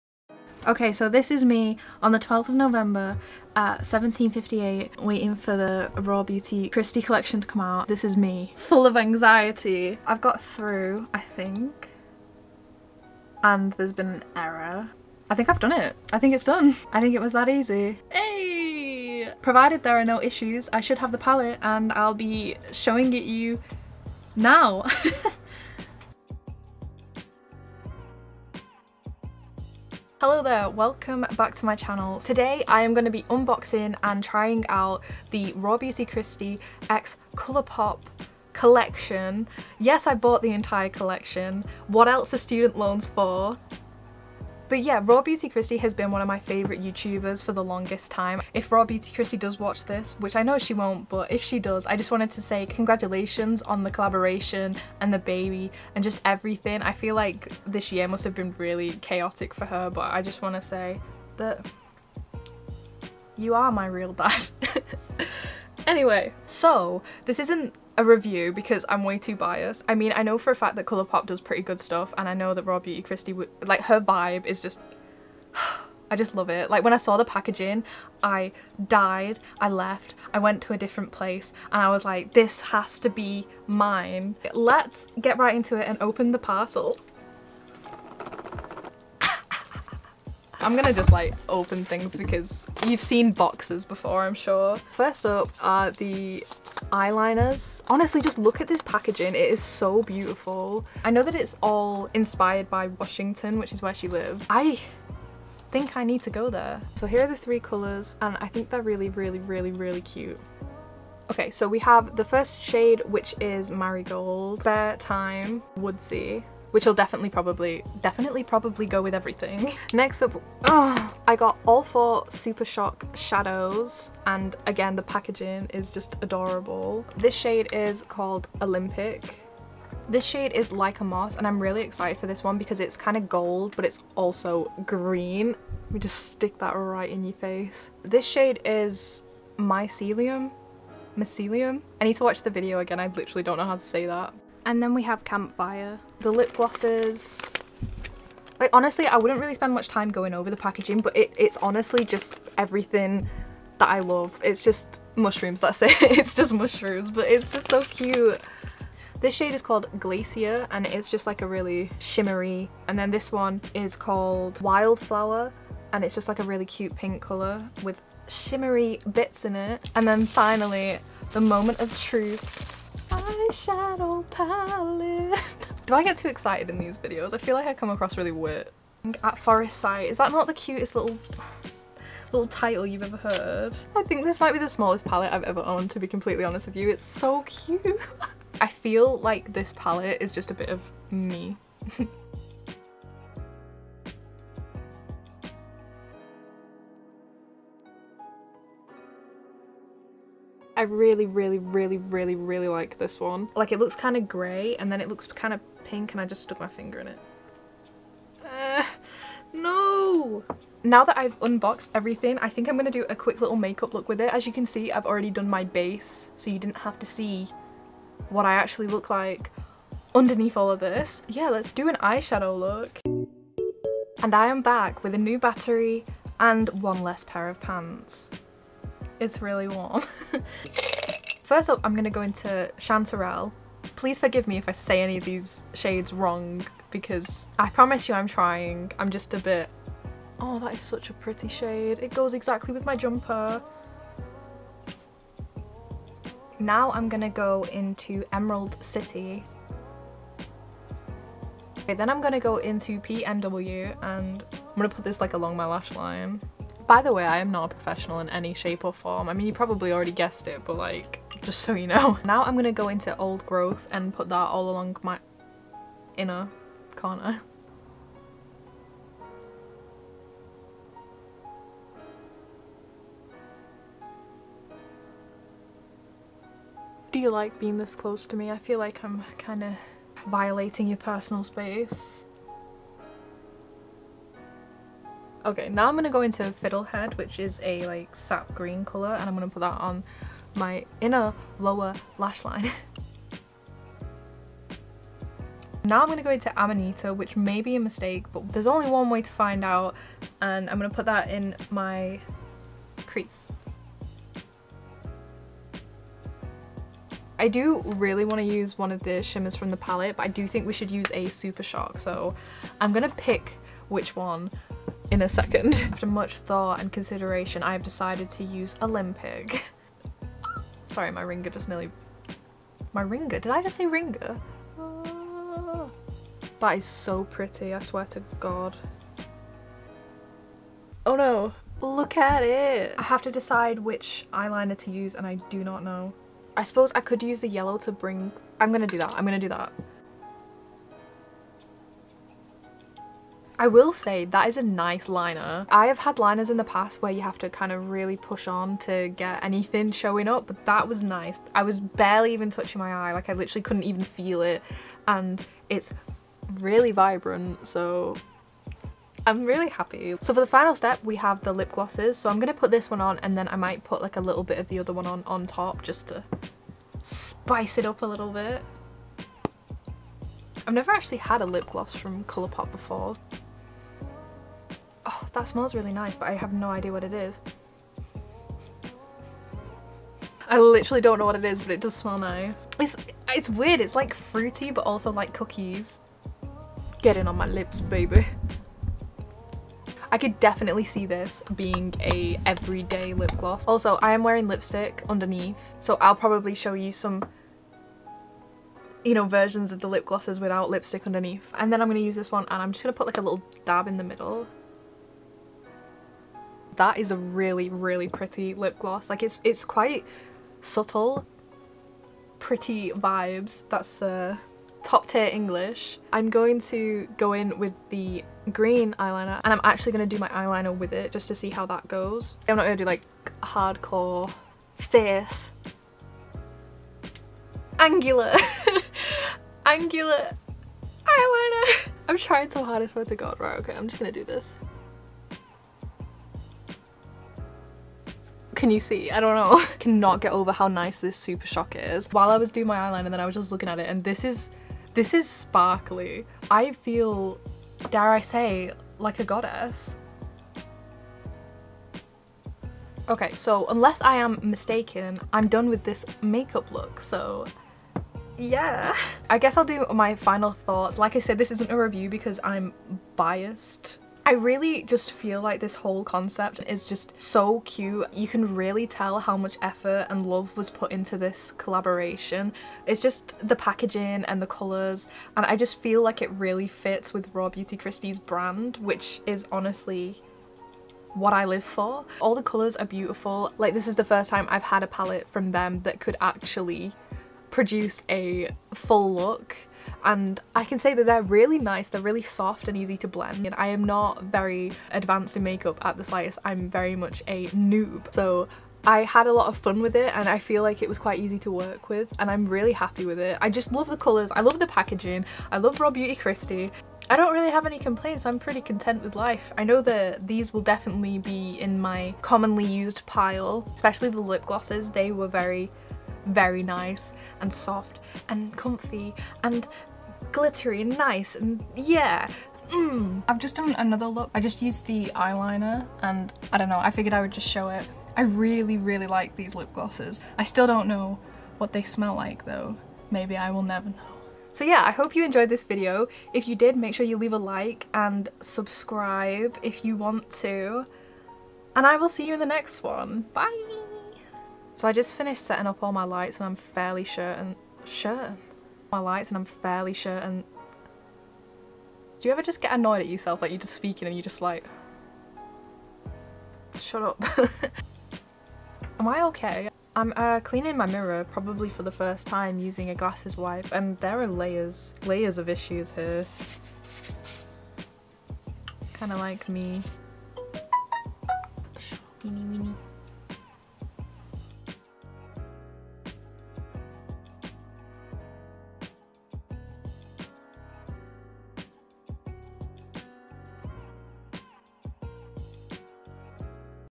Trying the RawBeautyKristi x Colourpop Makeup Collection | Spoiler: I'm in Love
I had a lot of fun trying out the new RawBeautyKristi x Colourpop makeup collection! I'm so happy with the finished looks (including the secret bonus makeup look at the end :O ) and I'm pretty sure this is my new favourite ❤️